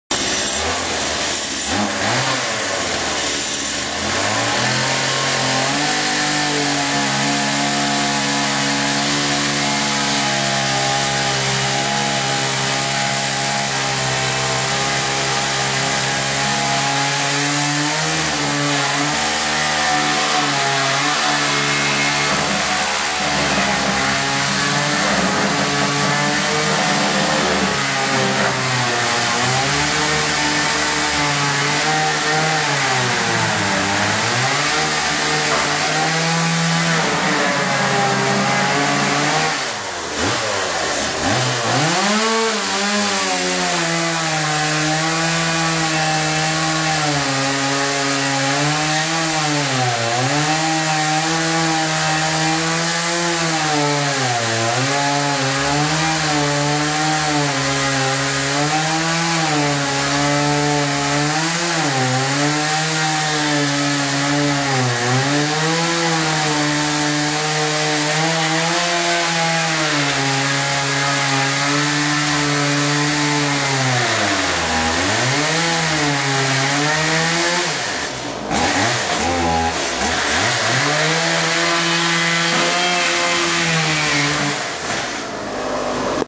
Bugsy Browns Gets A Facelift
Mike taking a chainsaw tothe bar counter as the refit begins